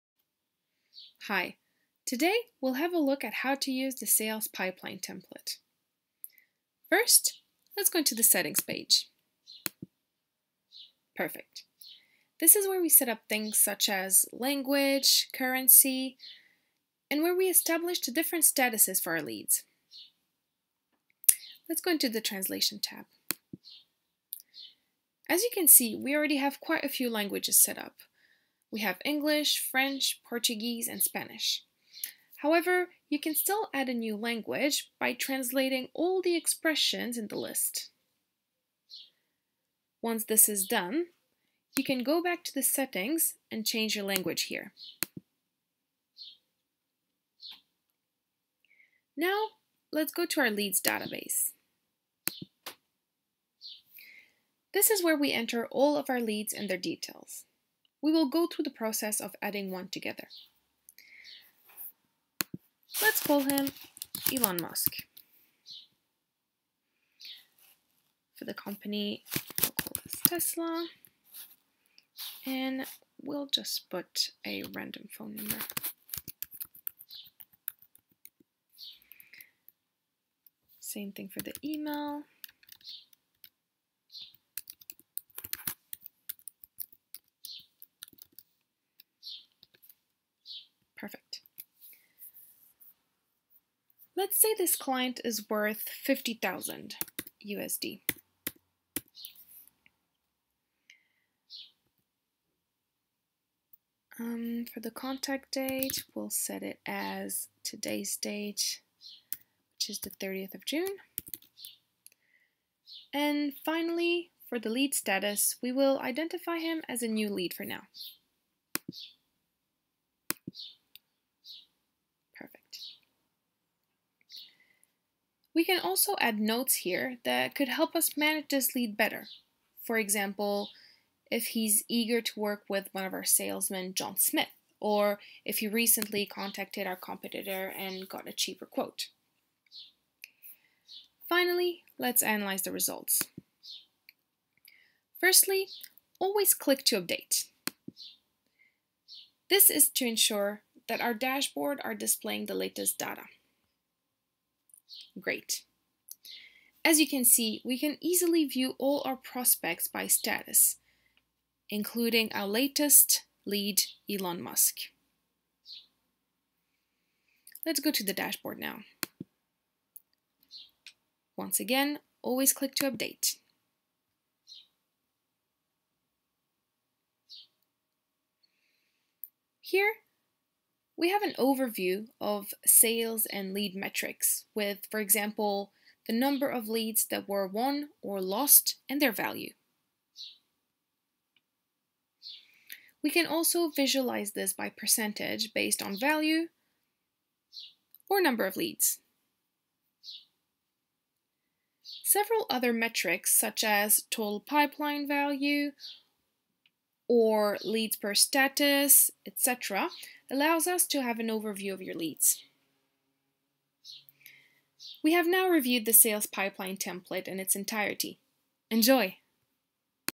Sales Pipeline Template - Walkthrough
The Sales Pipeline Template is an Excel Spreadsheet designed to help you visualize your sales process in the organization. The Sales Pipeline Template will allow you to track your sales activities and it will give you useful insights to grow your business.